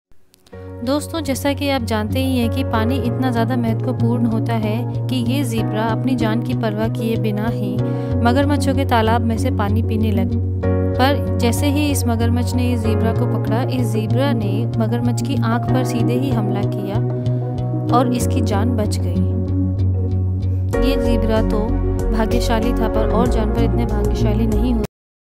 shorts crocodileAttackZebra crocodile zebra
Crocodile atack on zebra but he was lucky enough to escape